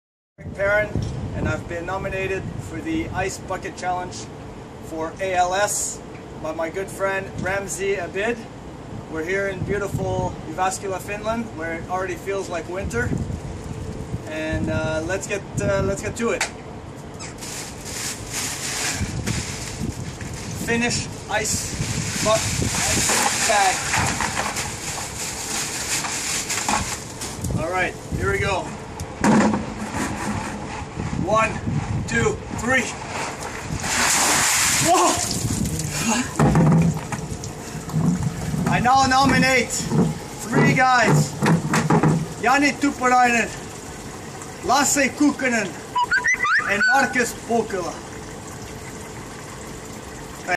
Eric Perrin - ALS Ice Bucket Challenge
JYP-kapteeni Eric Perrin vastasi ystävältään Ramzi Abidilta saamaansa ALS Ice Bucket-haasteeseen.